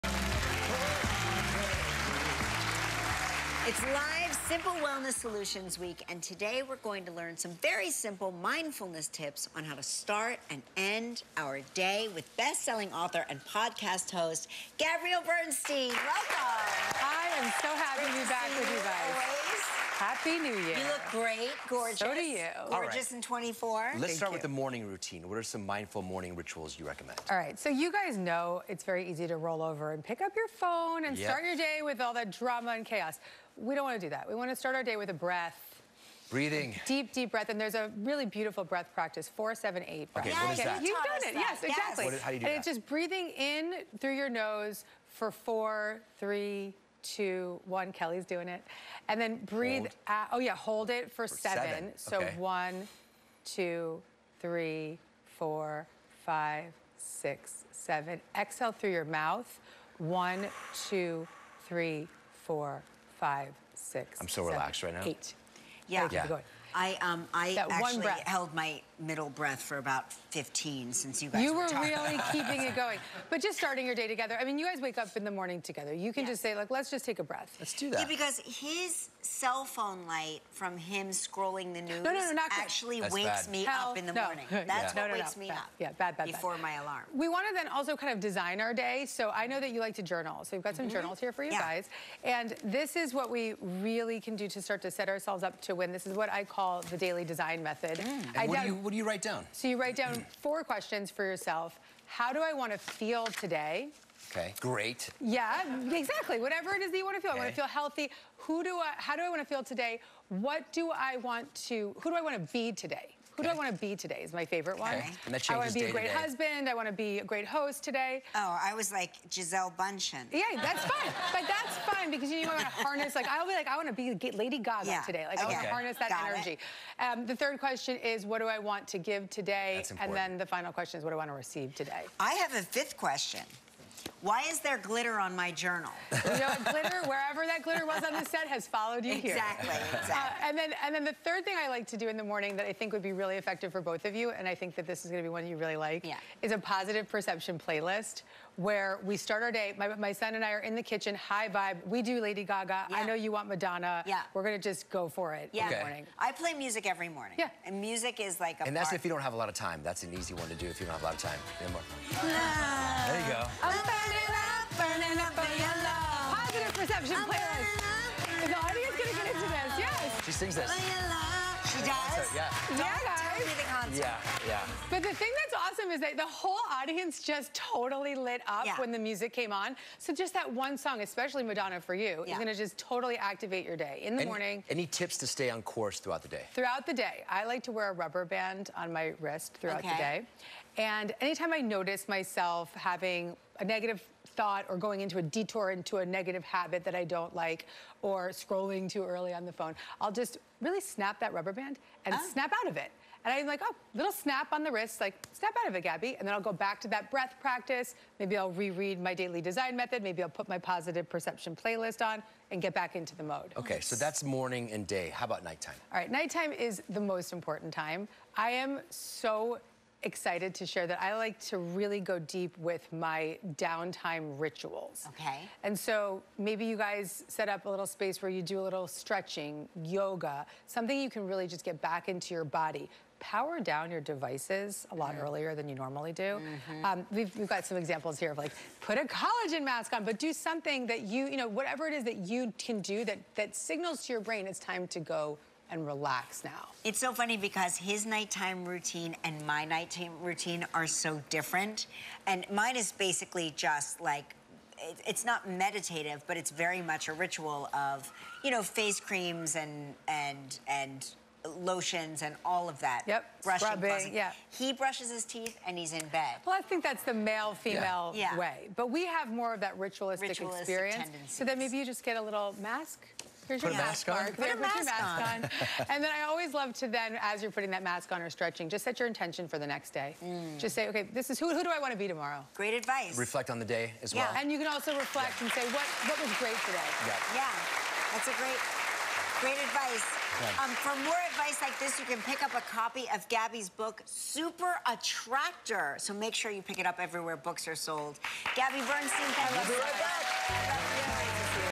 Strive for More in ‘24: Mindfulness Throughout the Day with Gabby Bernstein
Gabby Bernstein shares tips to start and end your day with mindfulness.